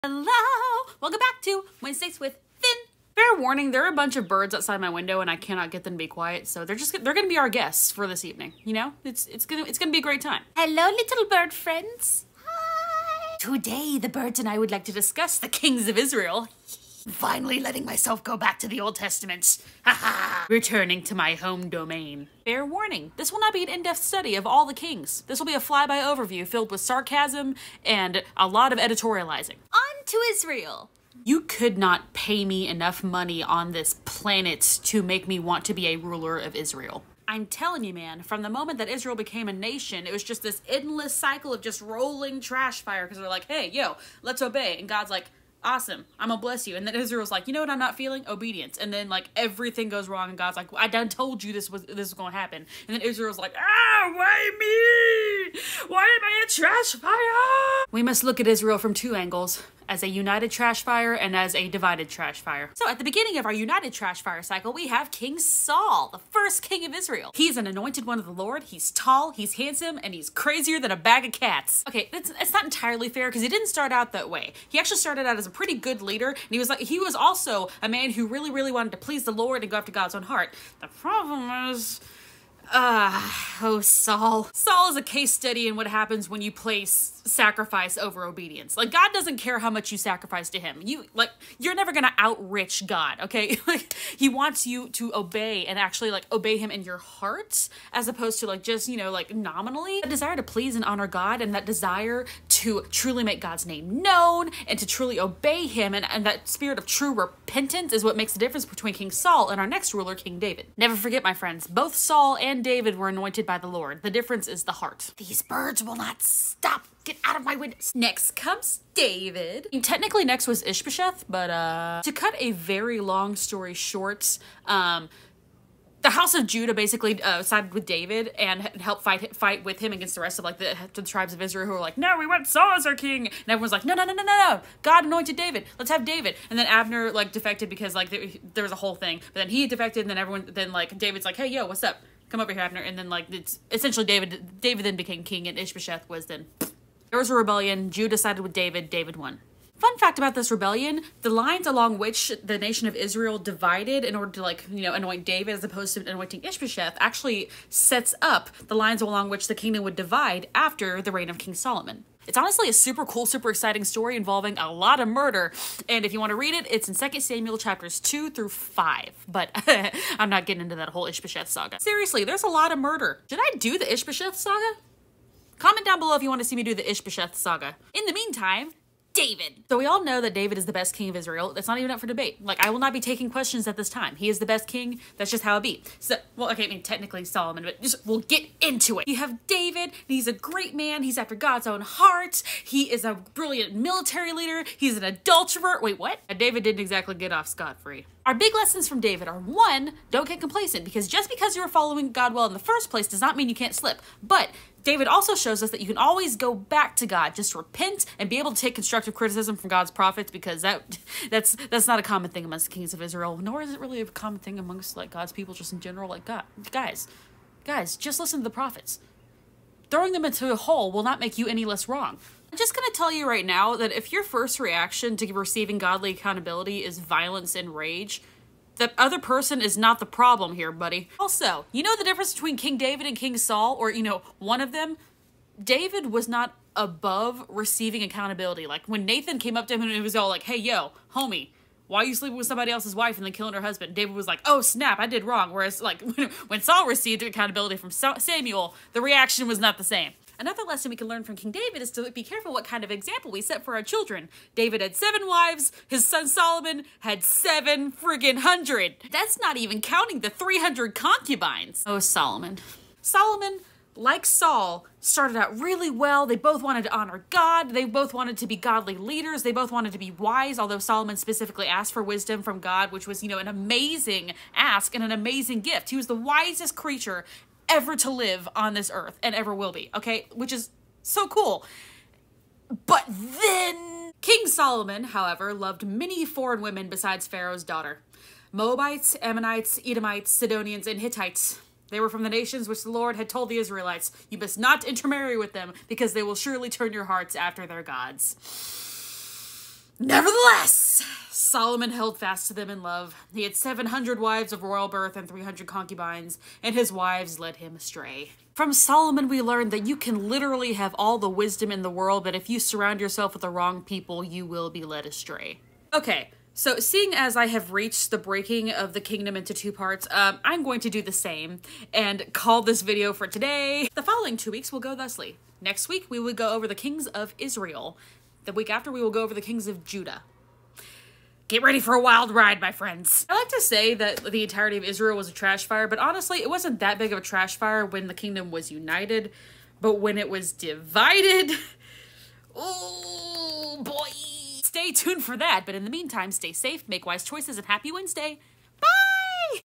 Wednesdays with Fin: Ep. 12
Hi friends! This is the first video in my series on the Kings. This video encompasses 1-2 Samuel  and 1 Kings 1-11.
It is not an exhaustive study, just a flyover roast of Saul, Ish-Bosheth, David, and Solomon.